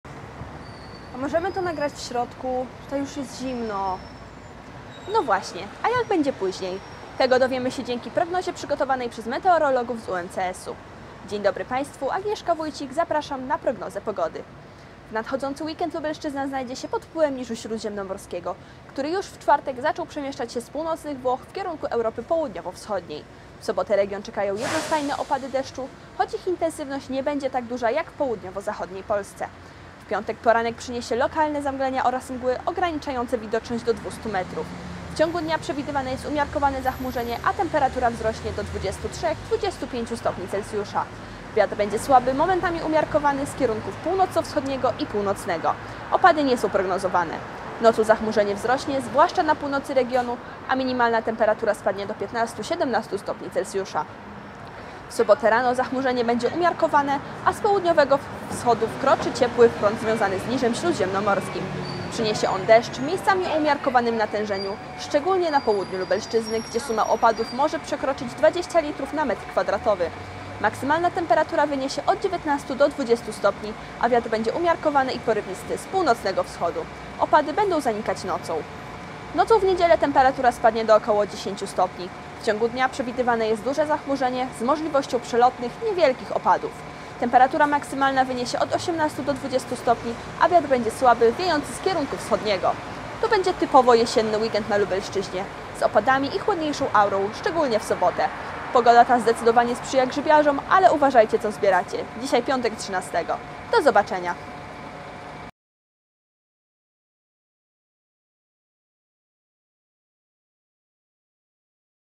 Czy to już jesień? - Weekendowa prognoza pogody
Zapraszamy do zapoznania się z weekendową prognozą pogody przygotowaną przez naukowców z Instytutu Nauk o Ziemi i Gospodarki Przestrzennej UMCS. Najbliższe dni przeanalizował meteorolog dr Krzysztof Bartoszek. Prognozę prezentuje Agnieszka Wójcik.